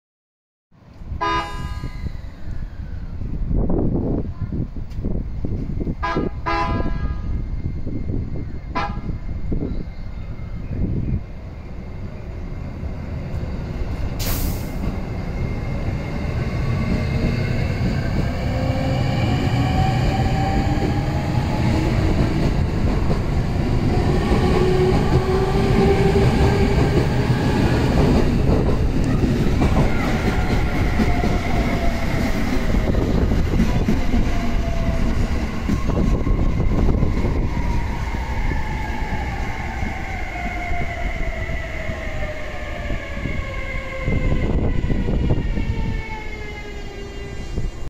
【国鉄色が似合ってる】e653系k70編成 フラッといわき巡り号 湯本発車
まさかひとりで湯本行く事になるとは……

ひとりで伊豆に行った実績もあるので そこまで不安じゃありませんでしたwww